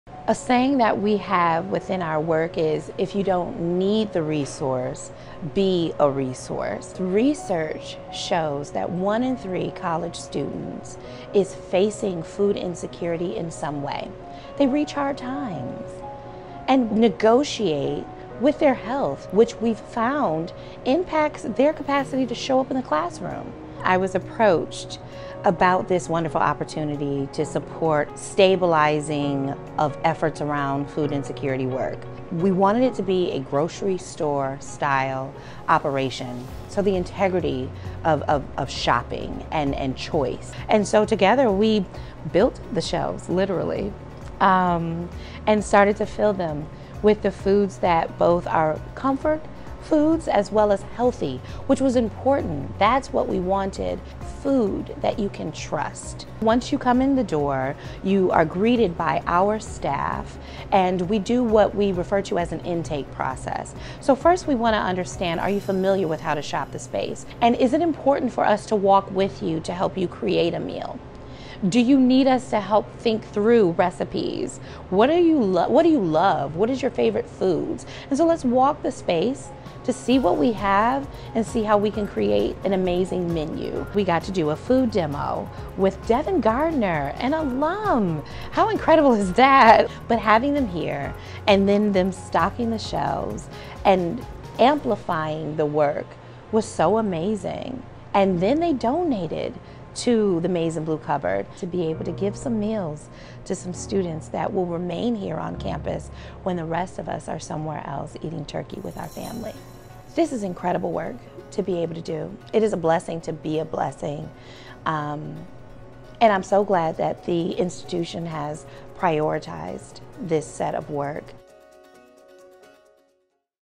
The Maize and Blue Cupboard at The University of Michigan
College students are experiencing food insecurity and lack of access to adequately healthy foods at an alarming rate. U-M’s Maize and Blue Cupboard is offering students immediate and comprehensive resources to remedy this. mbc.studentlife.umich.edu